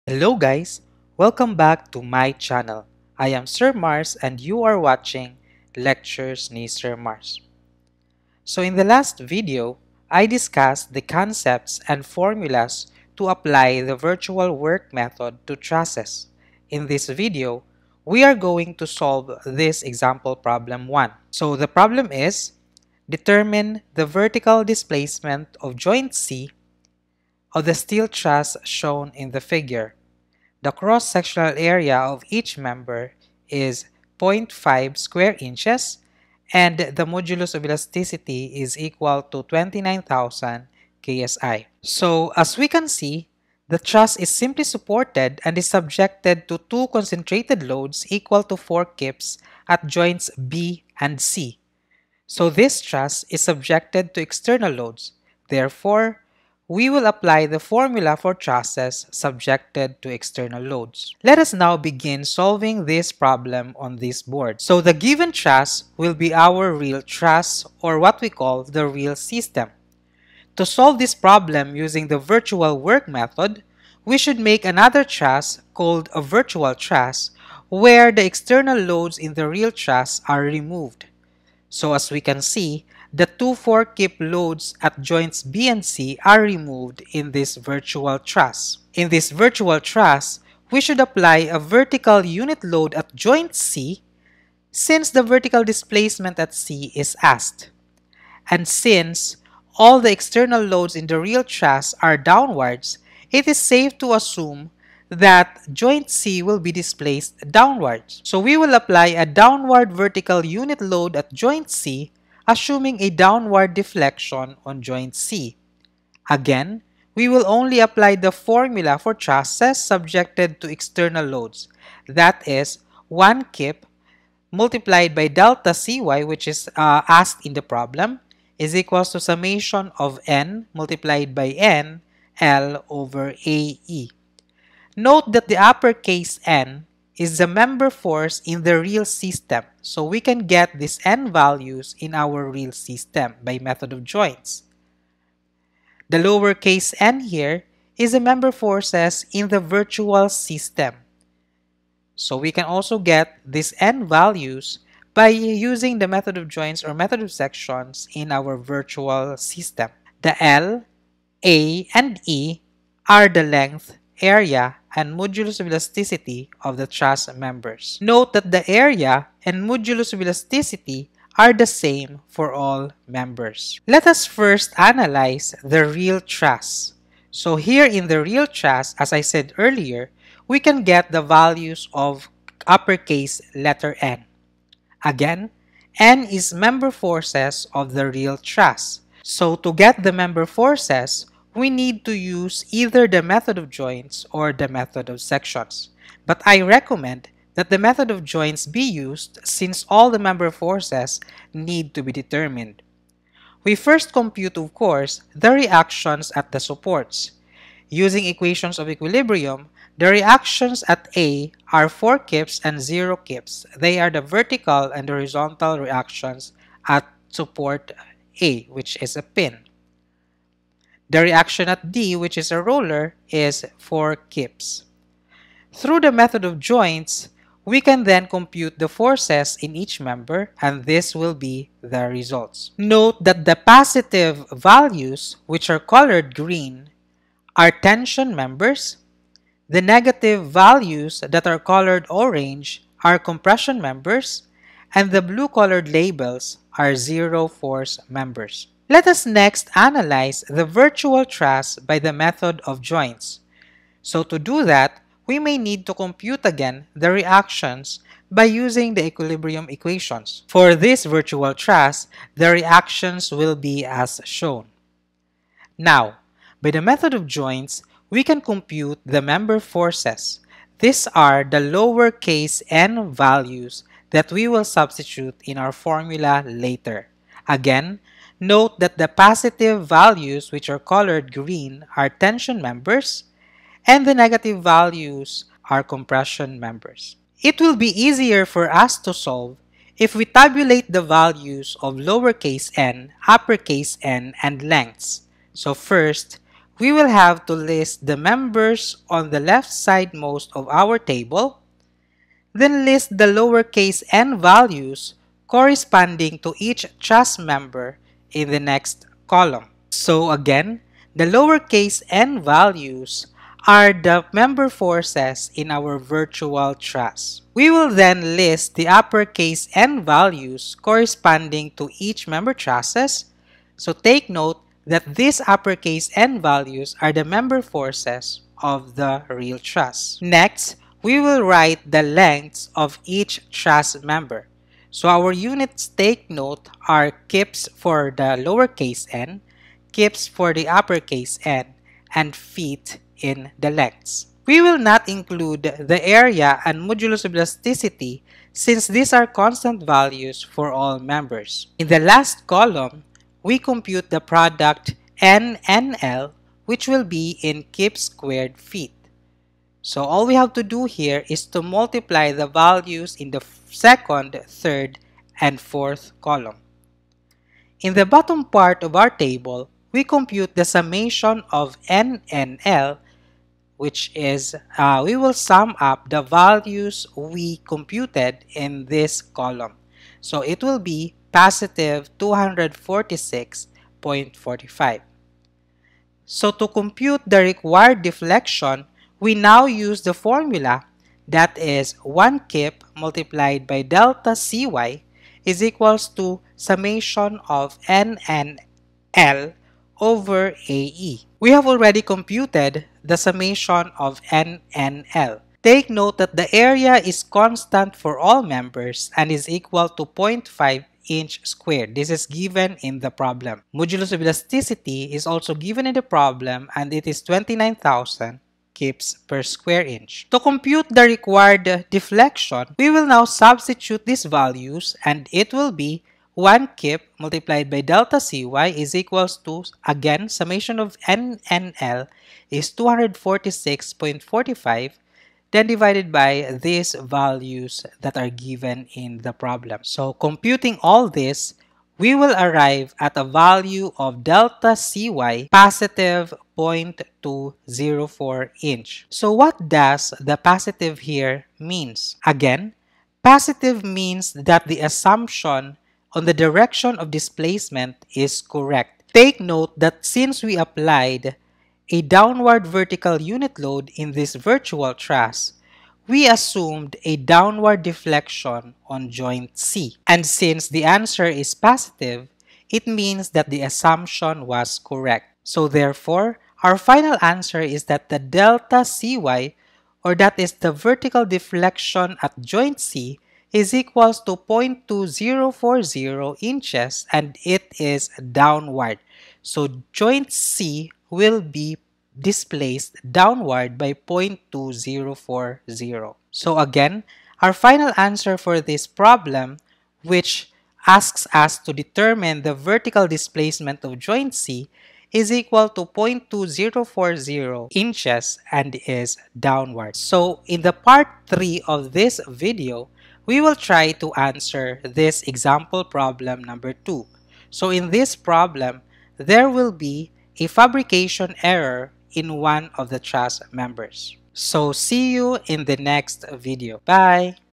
(2/3) Virtual Work Method for Trusses | Structural Theory
In this video, Sir Mars solves an example problem of using Virtual Work Method to truss subjected to external loads.

Or send to FB Page (Lectures ni Sir Mars)